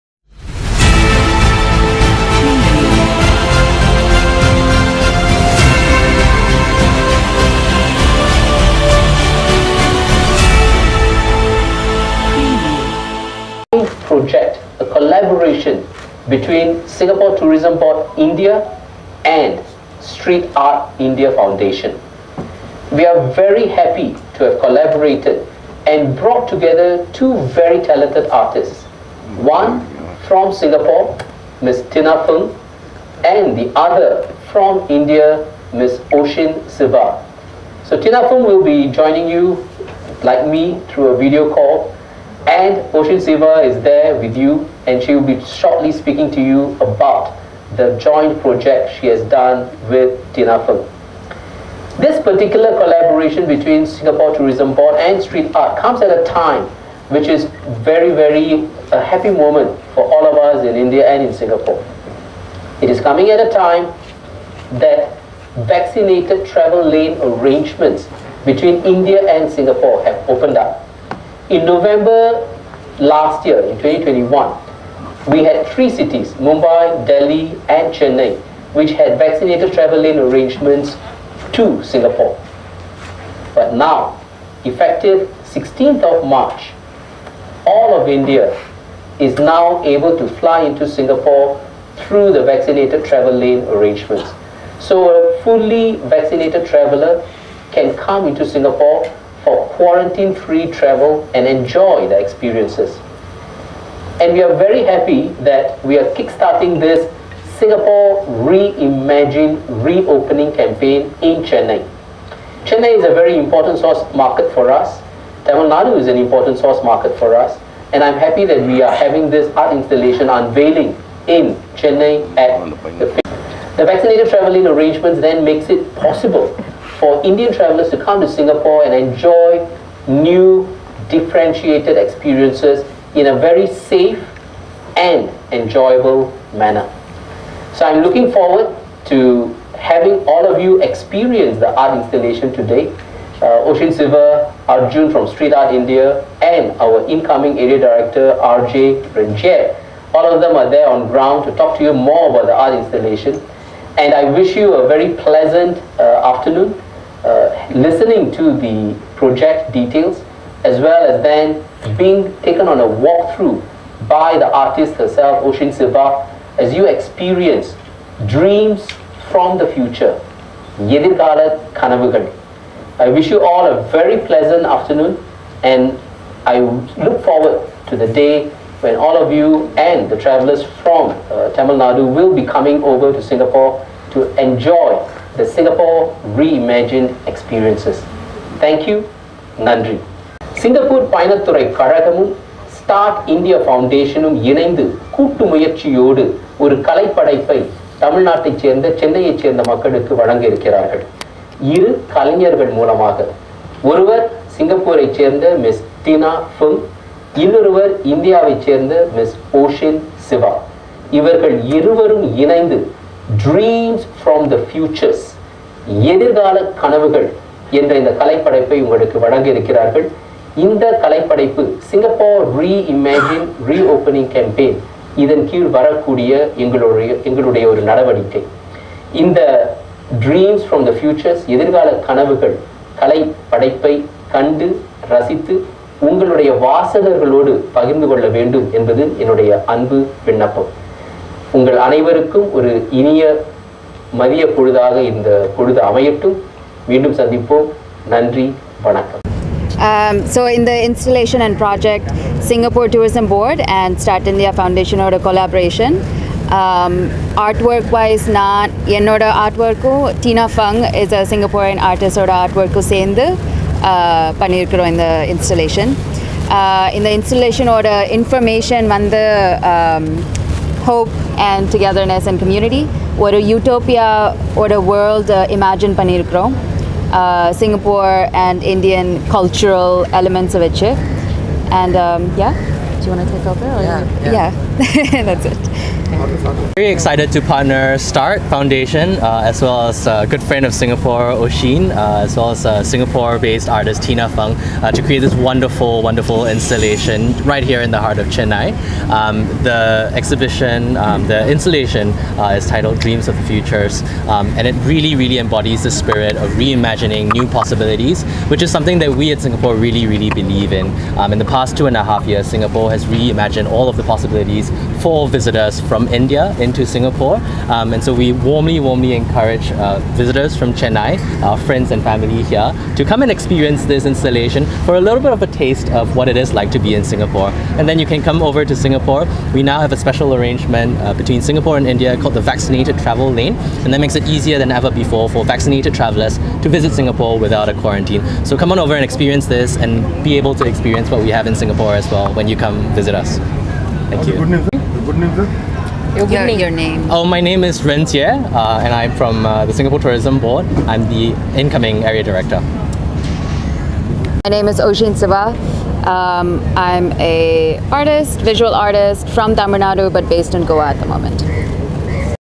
Singapore Tourism Board partners St+art India Foundation for an immersive art event part Singapore
Singapore is gearing up to welcome visitors from India with its SingapoReimagine campaign to get visitors to reignite their passion for travel through fresh and innovative experiences in Singapore. This follows the recent announcement of quarantine-free, two-way Vaccinated Travel Lane (VTL) between Singapore and all Indian cities for fully vaccinated travellers from 16 March 2022.
To drive visitation and inspire travel for Singapore, the Singapore Tourism Board (STB) has partnered St+art India Foundation (St+art) to create a large-scale outdoor, art installation titled “Dreams from the Futures”, a cross-culture artistic collaboration between Singapore-based artist Ms Tina Fung and Indian artist Ms Osheen Siva.

Inspired by the concept of Utopia, Dreams from the Futures/ எதிர்காலக் கனவுகள் (Ethirkaalak kanavugal) features the seamless amalgamation of traditional Tamilian and Singaporean elements – such as Peranakan motifs and classic mudras – juxtaposed with futuristic elements like cosmic portraits to showcase inclusivity, coexistence, and a connection between past and future.

This multi-layered installation invites Chennai audiences to imagine new and expansive futures, in line with the theme of SingapoReimagine as visitors can look forward to new, unexpected and innovative experiences in Singapore, alongside enhanced hygiene and safety measures. The installation will be on display for three (3) weeks at Phoenix Market City, Chennai. The latest installation continues the series of cross-cultural artworks that STB and St+art have jointly unveiled at iconic locations like Sassoon Docks, Mumbai, Lodhi Colony, Delhi and Little India, Singapore, all brought to life by the spirit of the vibrant island destination’s brand, Passion Made Possible.

By weaving familiar elements from both cultures with futuristic imagery, the installation aims to reimagine the known and transport us through a journey of possible dreams. The diverse face silhouettes serve as a gateway into the work. As audiences walk through them, they are met by fellow visitors to share a sociable and explorative space. From the entrance onwards, the visitors are encouraged to consider the human connections we make as we cross roads with new cultures and people.

Speaking about the artistic collaboration, Mr. GB Srithar, Regional Director- India, Middle East & South Asia (IMESA), STB, said, “As Singapore reopens its borders cautiously and gradually for international travel, the country is ready to welcome back Indian travellers with renewed experiences and seamless safety standards. Together with St+art India Foundation, our ‘SingapoReimagine’ activation in the beautiful city of Chennai celebrates the Tamilian history and cultural connection between both the countries through the expression of art. Going forward, we aim to elevate the campaign to other parts of the country to bring out the cultural commonalities, and also give a glimpse of the reimagined experience that visitors to Singapore can enjoy.”
Giulia Ambrogi, Curator, St+art India Foundation, said, “I’m delighted to have had the opportunity of collaborating with such talented artists, thanks to the continuous support of The Singapore Tourism Board. Art can be extremely relevant in such difficult times and this installation aims at being a message of hope and of coexistence, looking at the future as something we can all reimagine together beyond borders. The immersive installation works as a crescendo, converging into the well as a final vision and epiphany to start from, in thinking about expansive inclusive futures.”
Talking about the art installation, Singaporean artist, Tina Fung, said, “I wanted to create an environment which parallels desire for experiences that are thought-provoking, fun and immersive. The work is not only social media worthy but most importantly engages, educates and supports the overall narrative and connection.”

Indian artist, Osheen Siva, added, “It has been a joy to work on this project, as it is an extension of the themes that I often like to explore in my own art practice - solidarity, utopia, connection and togetherness. I've loved working with Tina and her studio as they beautifully and seamlessly brought our ideas into the colorful, engaging and interactive physical realm.”